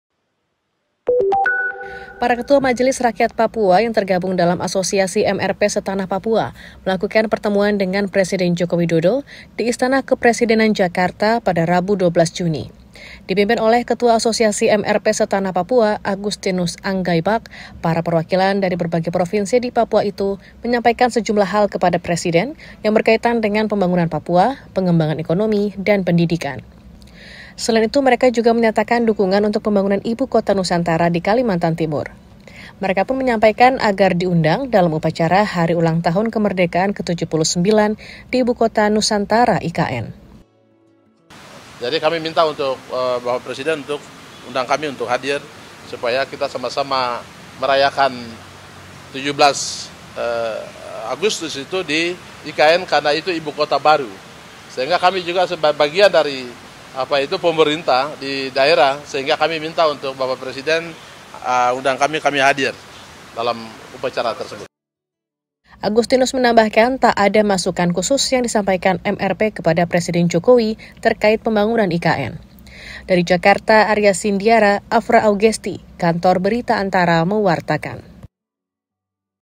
Bertemu Presiden, Majelis Rakyat Papua sampaikan dukungan untuk IKN
ANTARA - Para ketua Majelis Rakyat Papua melakukan pertemuan dengan Presiden Joko Widodo di Istana Kepresidenan Jakarta pada Rabu (12/6). Ketua Asosiasi MRP Se-tanah Papua Agustinus Anggaibak, menyampaikan saat dijumpai usai pertemuan tersebut, bahwa pihaknya menyampaikan dukungan terhadap pembangunan Ibu Kota Nusantara. (Aria Cindyara/Afra Augesti/Soni Namura/Farah Khadija)